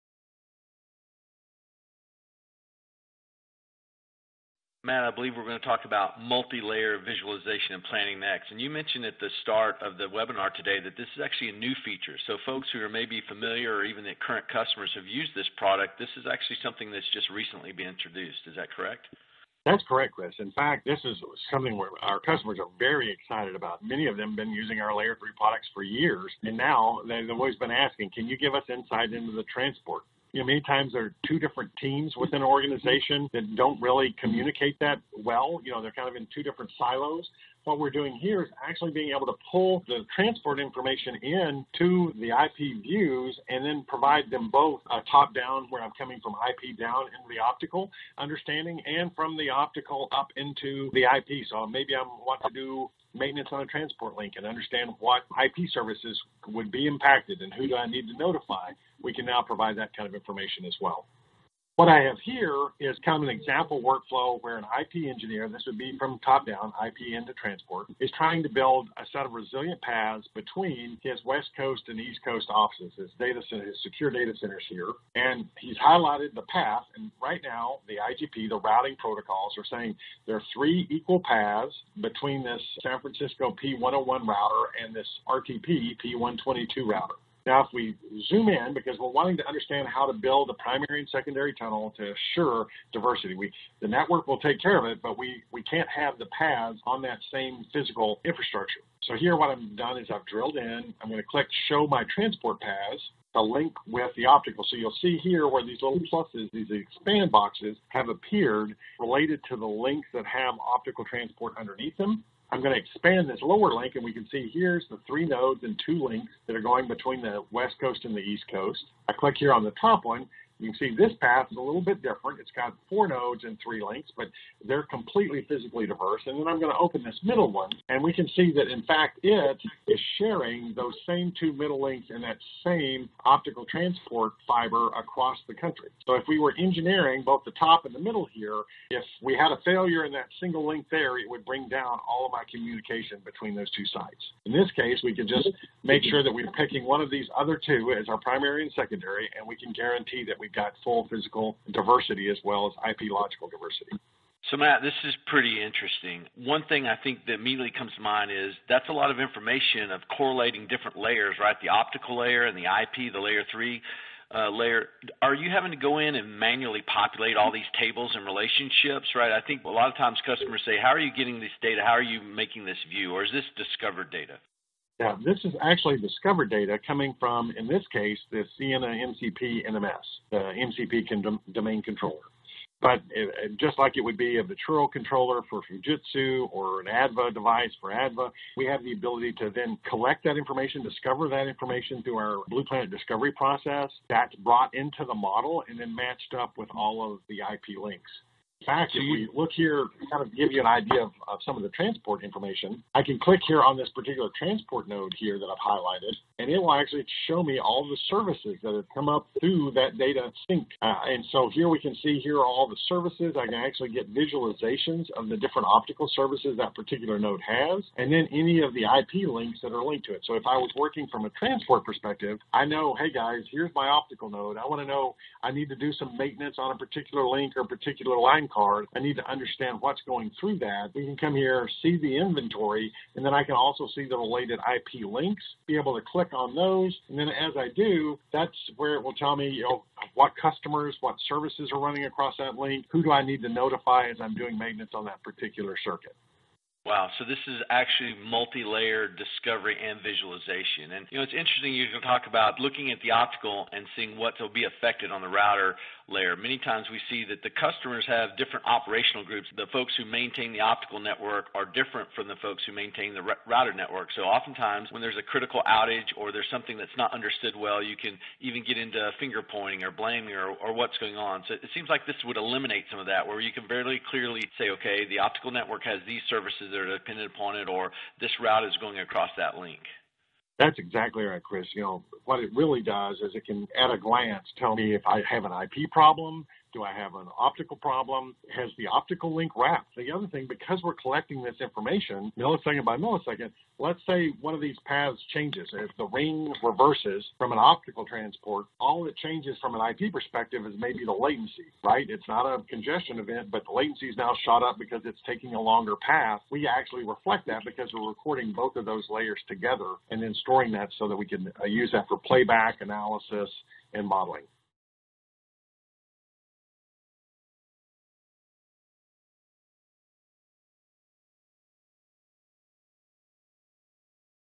Visualización y planificación de múltiples capas
El soporte de transporte e IP a menudo lo realizan distintos equipos que cuentan con información de la red en silos y no tienen la capacidad de correlacionar la planificación o solucionar problemas con las redes de transporte y IP.  Esto puede tener consecuencias indeseadas cuando se realizan actividades de mantenimiento.

Vea esta breve demostración para conocer cómo Route Optimization and Analysis de Blue Planet recopila en tiempo real los datos de la red descubiertos, correlaciona esta información y visualiza la capa de transporte y la capa IP en una vista única fácil de usar que proporciona al usuario visibilidad desde una sola fuente de servicios y clientes IP específicos y de servicios ópticos y nodos de transporte específicos.  Nunca más planifique, mantenga o resuelva a ciegas.